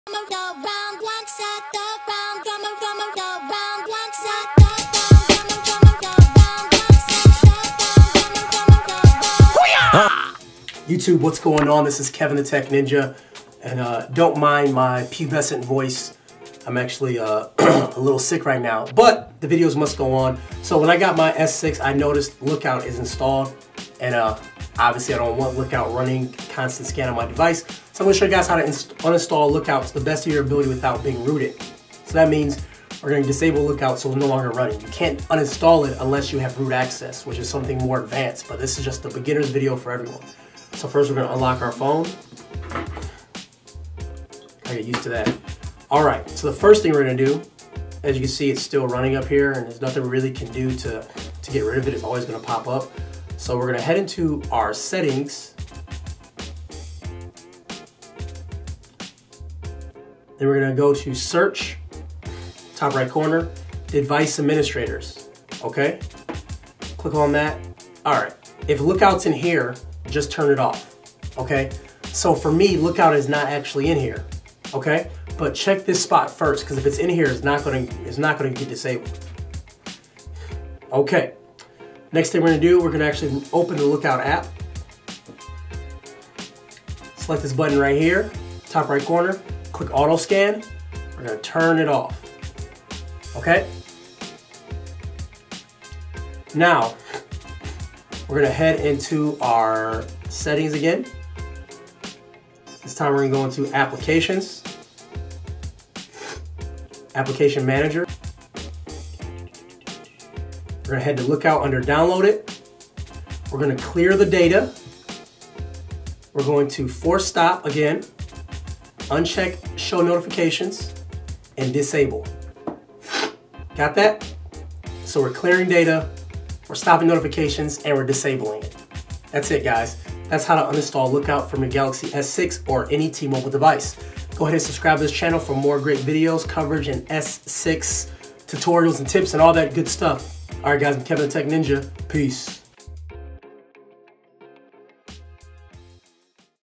Remove Mobile Lookout On Galaxy S6 or S5
Quick video on how to Remove Mobile Lookout on you T Mobile Galaxy S5, or S6.